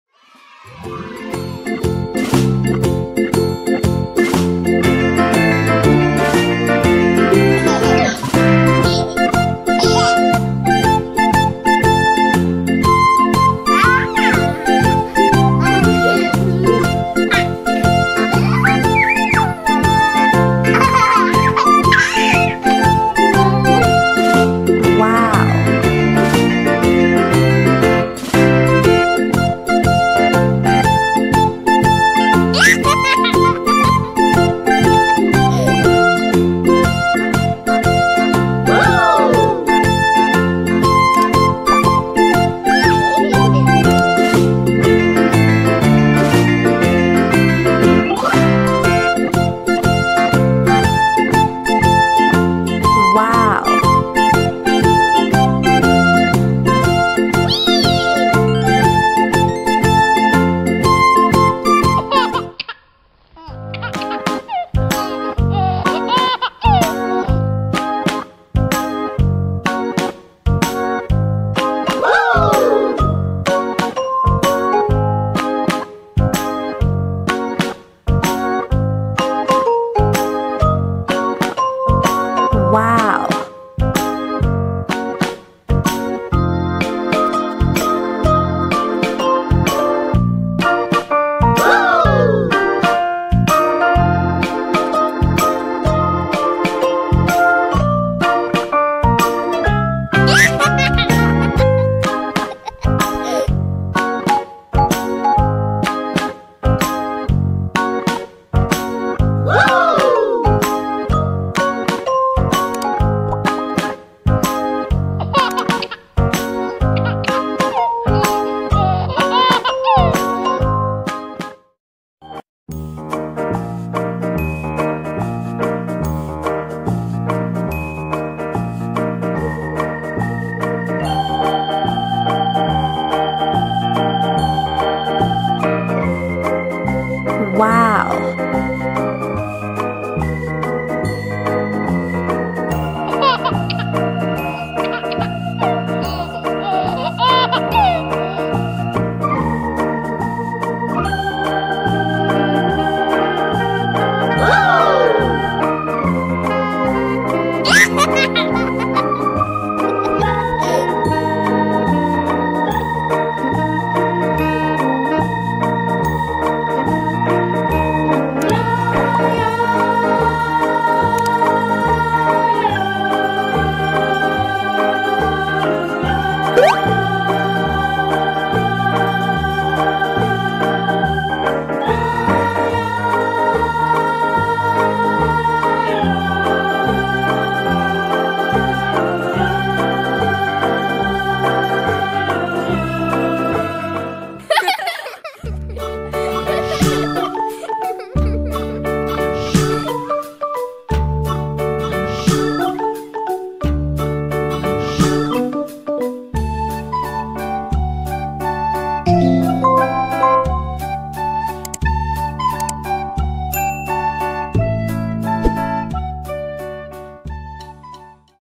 Colorful House Drawing For Kids, Painting and Coloring For Kids, Toddlers
Learn how to draw Rainbow house Drawing For kids Painting and Coloring For Kids Toddlers step by step complete Tutorial Let's draw together and have fun.